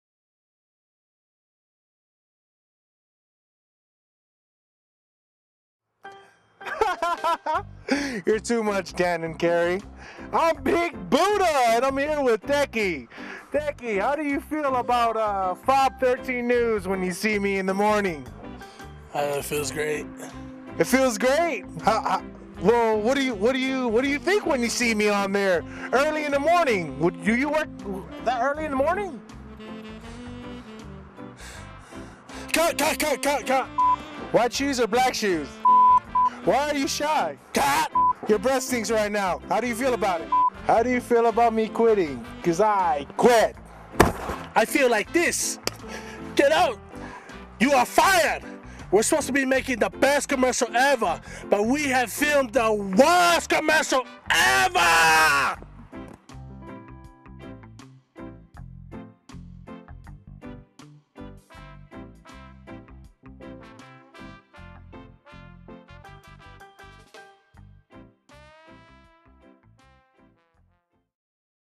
Laughing Samoans in Utah promo part 3 for the P.A.L.E. Foundation - Big Budah and Teki Spoof
The Laughing Samoan's will be in Utah as well as Adeaze
Featuring up and coming sensation Teki
to get more info on how to purchase tickets.